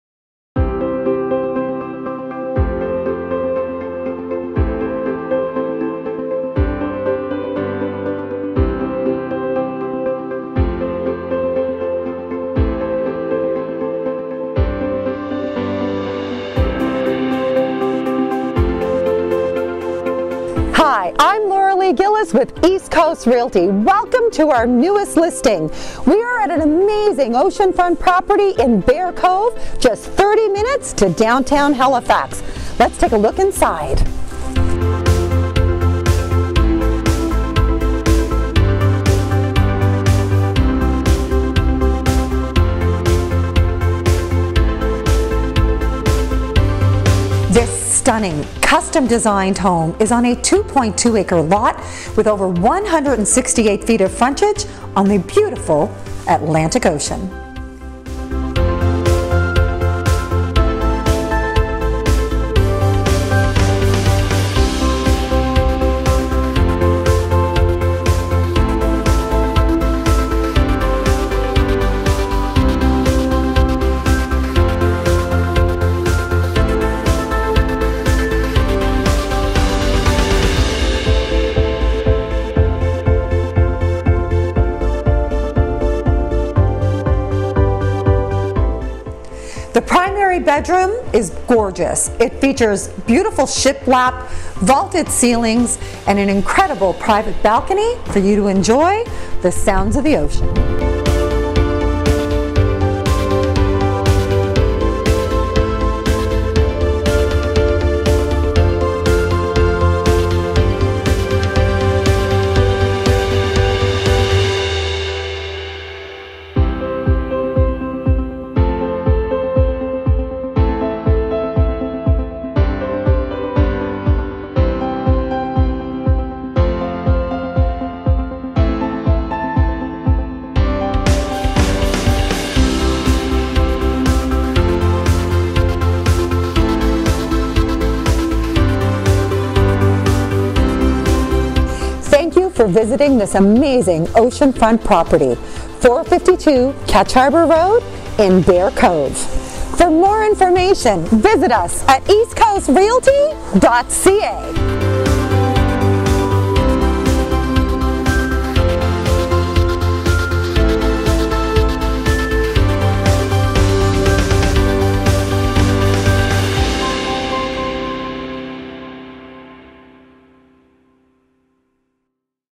Stunning Nova Scotia Oceanfront Property 452 Ketch Harbour Rd
Stunning custom designed home with 168 feet of coastline on the beautiful Atlantic Ocean!
Luxurious living awaits at this address, welcome to 452 Ketch Harbour Road!
Nestled on a private 2.2 acre lot in the desirable “Breakers at Bear Cove”, this 1 year young home was thoughtfully designed to capture stunning ocean views from all principal rooms. Boasting an array of beautiful finishes & open concept living space with an abundance of natural light, this home is your dream come true!
The main level features a great room with vaulted ceilings, a stunning propane fireplace a generous kitchen with coffered ceiling, beautiful cabinetry and Arabescato granite on the island and a dedicated dining area offering spectacular views and access to the patios that flank this great space.
On this level you will also find an amazing flex space tucked away behind the kitchen area that is perfect for a den or tv room, an office space as you enter (with closet), pantry, powder room and access to the attached 1.5 garage and storage area. The upper level features a spacious primary bedroom accented with shiplap & vaulted ceiling and your own private balcony! The ensuite is sure to impress with a walk-in shower, double vanity, toiletry closet, and jetted tub perfectly positioned for you to enjoy the scenery as you soak.Two more bedrooms, convenient laundry room and full bath complete this level. Highlights include gorgeous lighting, high end fixtures, high quality vinyl plank flooring and polished porcelain tile throughout the home, ducted heating and cooling, granite and quartz countertops, floor-to-ceiling windows, propane fireplace. Two outdoor patios to enjoy - the larger one is accessible from the great room and dining room and the smaller, private patio from the dining area-a beautiful home to host guests for parties or wedding. The fenced backyard is perfect for kids and pets. Scenery and serenity live here.You should too.